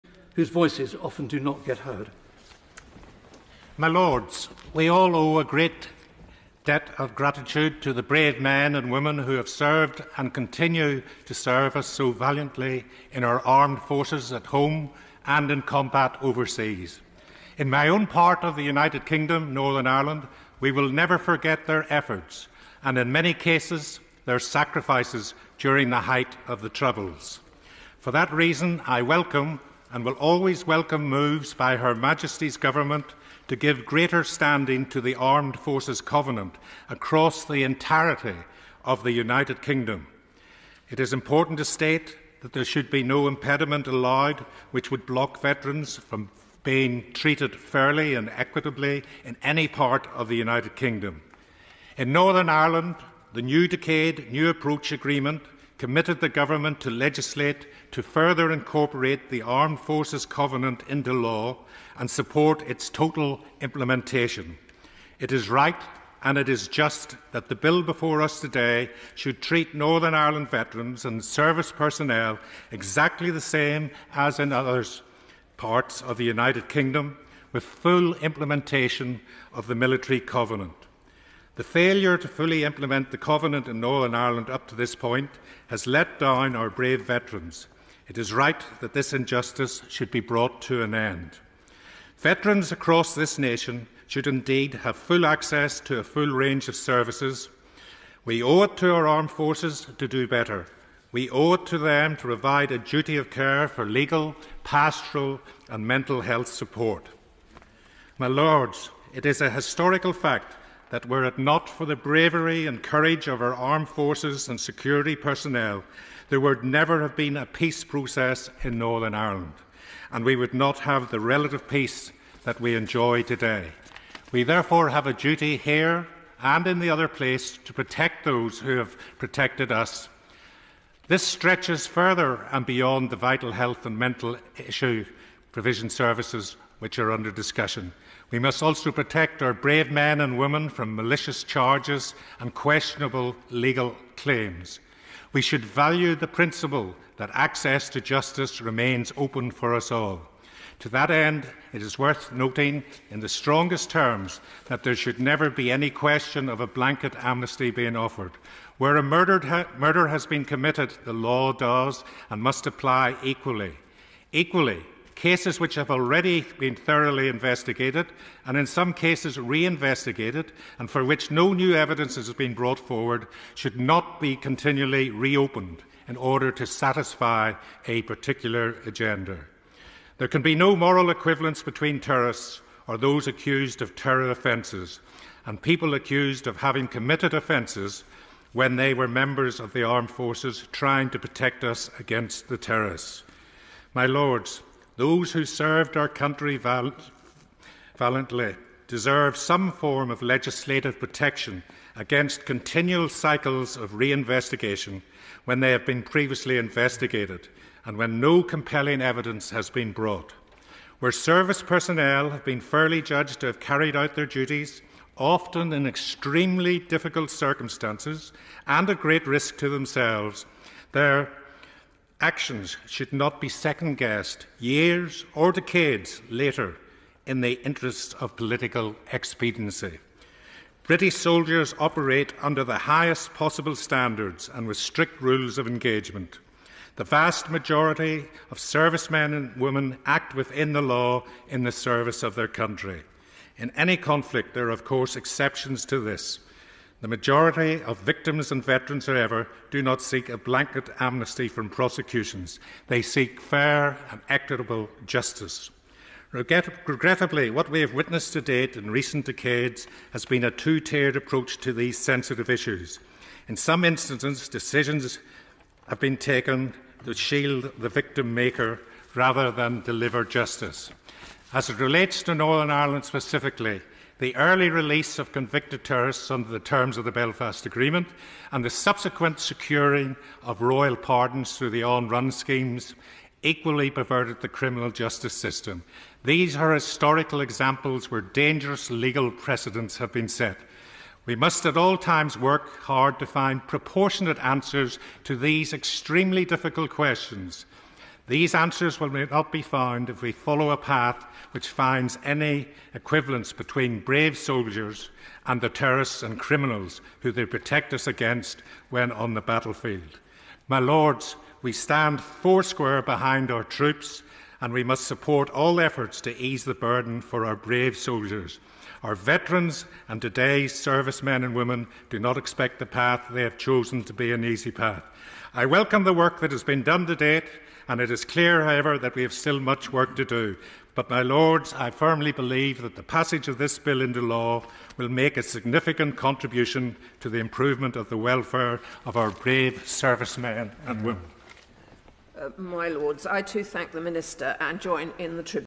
Lord Browne speaks on Armed Forces Bill
DUP Peer Lord Browne of Belmont speaking in the House of Lords during the Armed Forces Bill debate on 7th September 2021.